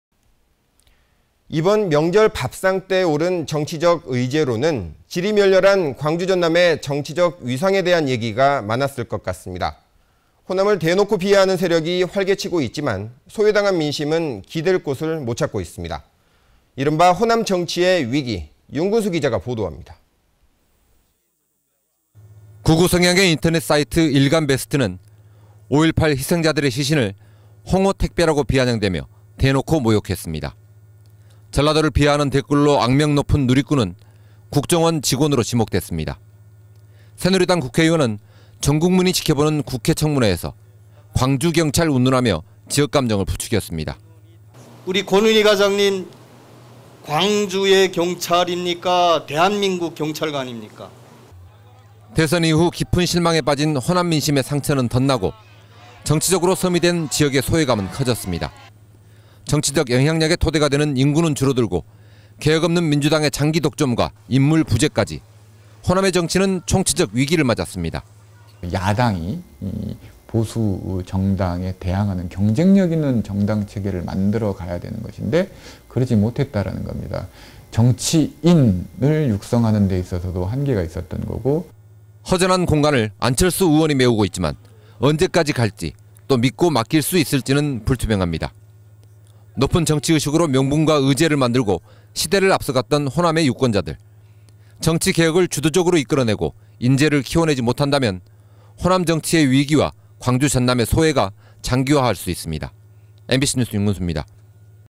HD [광주MBC뉴스] 위기의 호남 정치
이번 명절 밥상에 오른 정치적 의제로는 지리멸렬한 광주전남의 정치적 위상이 많았을 것 같습니다.

호남을 대놓고 비하하는 세력이 활개치고 있지만 소외당한 민심은 기댈 곳을 못찾고 있습니다.

이른바 호남 정치의 위기. 윤근수 기자입니다.